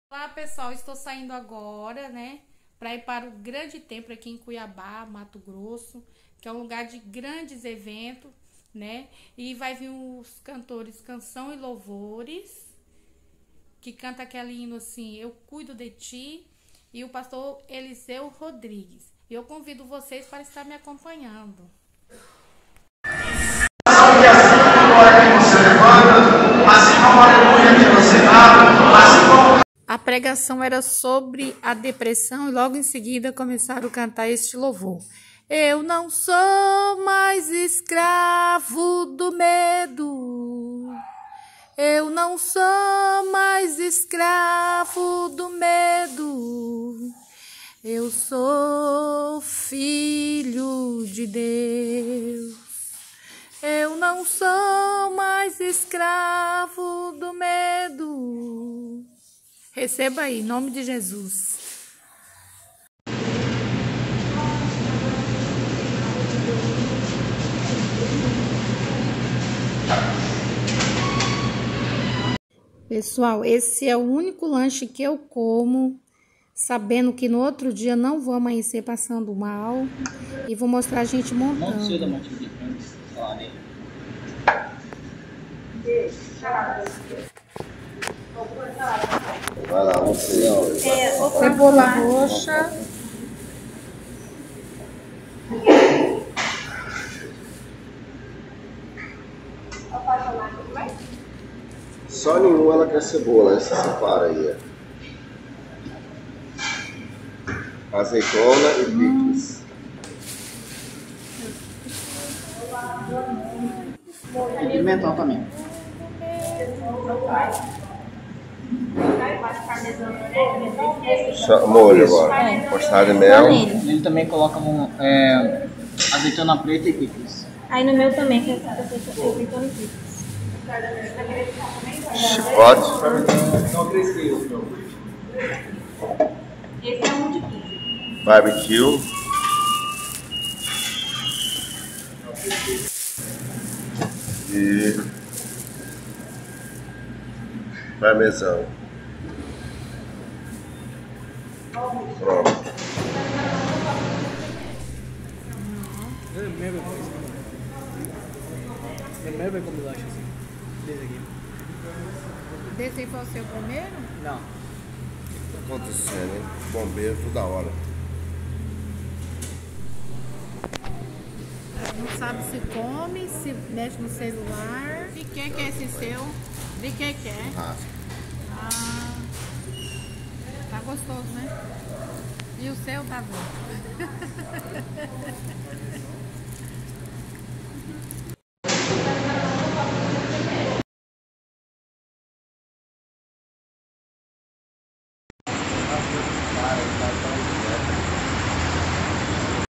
VLog: UMADECRE a igreja cheia de jovens! Depois
Olá! sentimos a presença maravilhosa Deus no grande templo não existe lugar melhor do mundo do que está na presença de Deus! o mundo precisa disso.Vlog mostra a multidão adorando a Deus e também o nosso lanche depois do culto🙏🏻